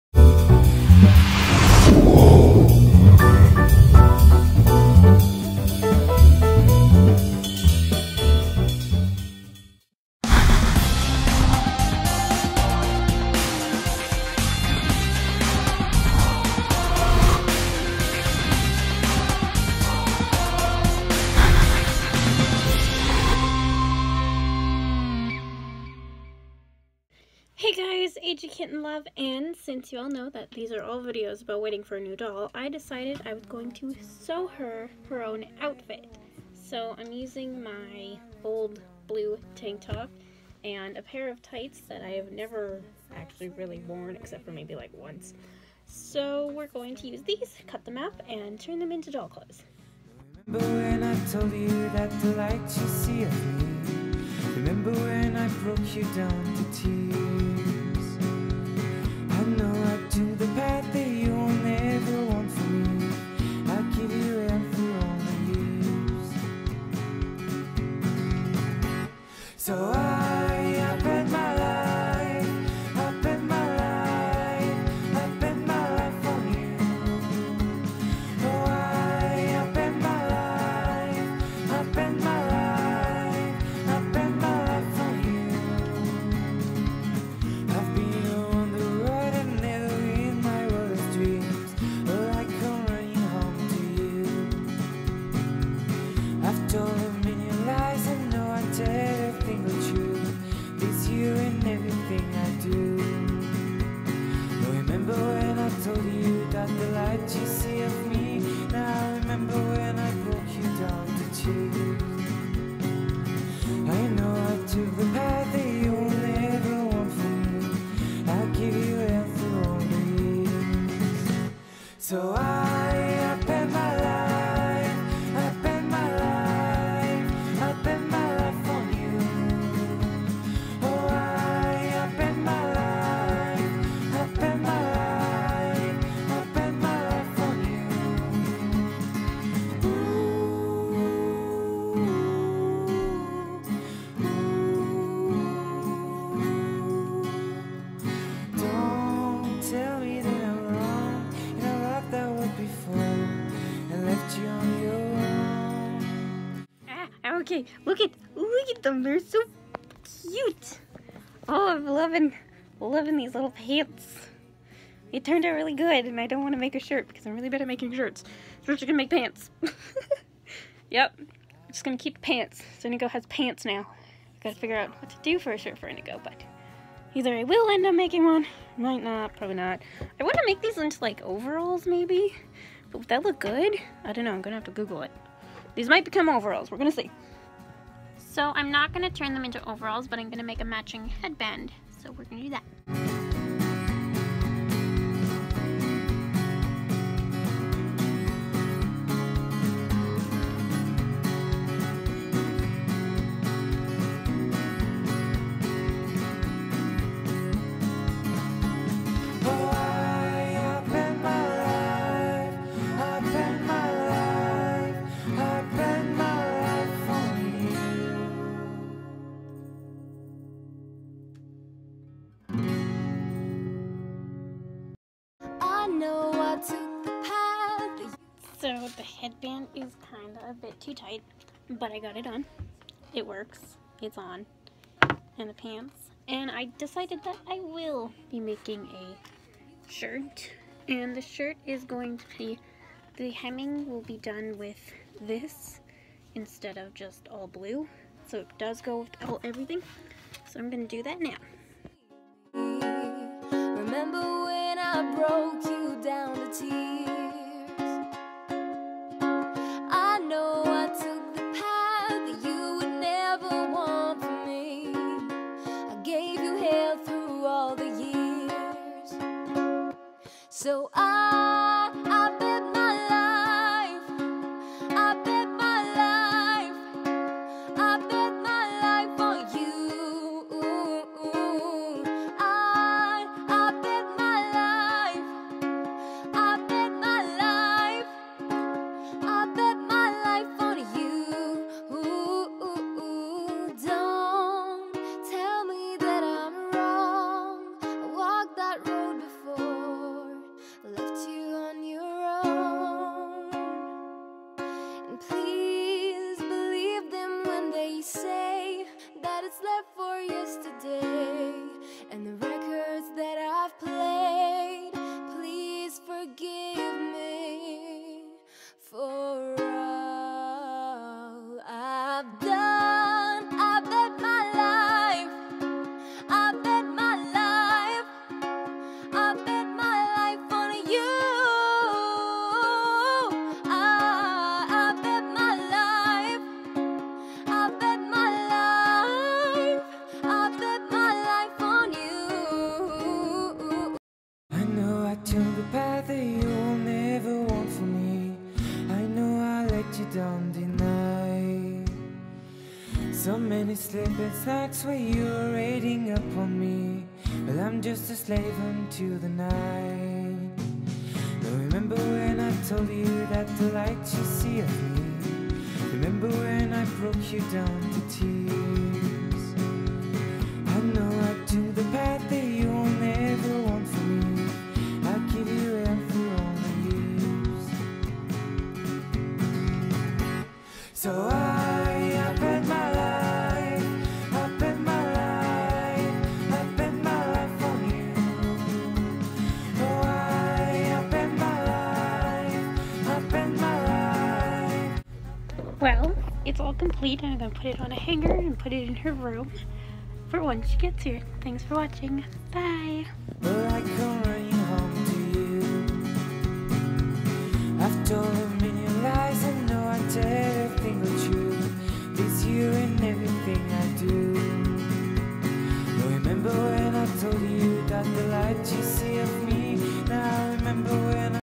Making a doll outfit || Waiting For a New Doll || Day 10
I have ordered a doll from @universedolls I’m so excited for when she gets here. I have decided to upload a video everyday until my new doll arrives.